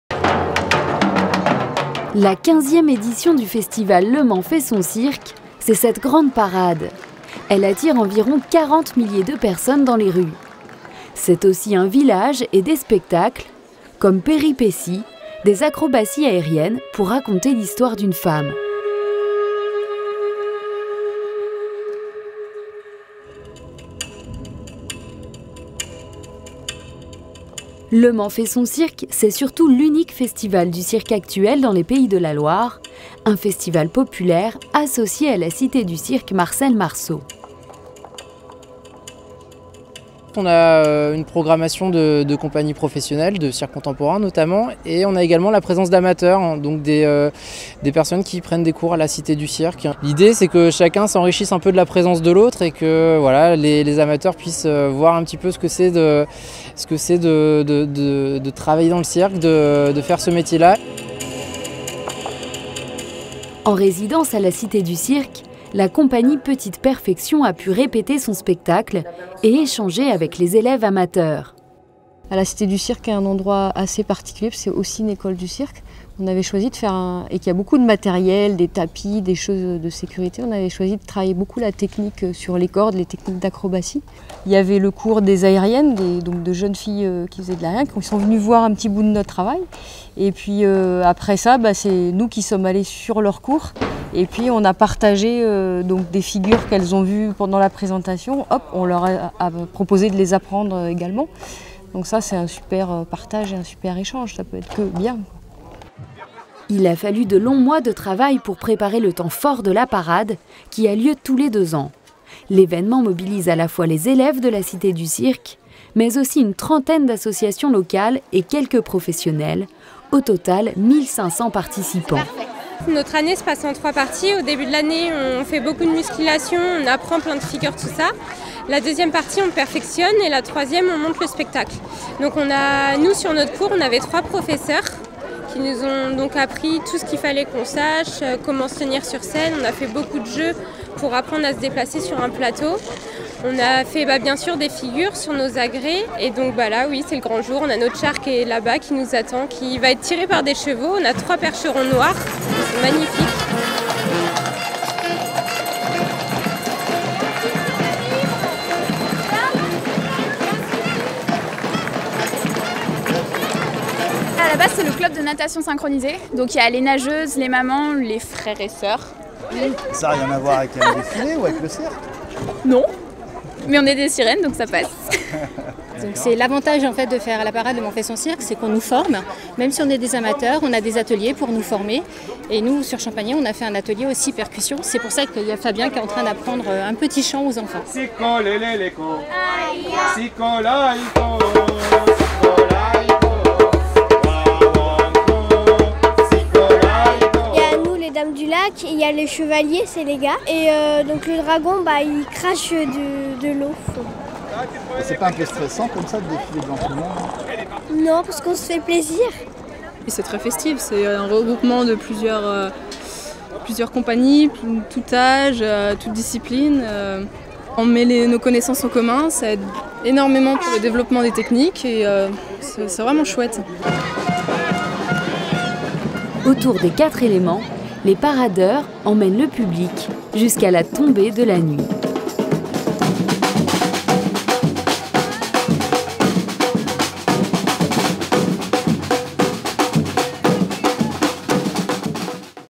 Le Mans fait son cirque : pros et amateurs partagent et paradent !
Professionnels et amateurs préparent de longue date le point d’orgue du festival Le Mans fait son cirque : la grande parade ! Les amateurs, ce sont les associations locales, mais également les jeunes formés à la Cité du Cirque Marcel Marceau, aux côtés d’acrobates confirmés.  Une cohabitation propice au partage et à l’apprentissage…

Réalisation : Atlantic TV © Région des Pays de la Loire.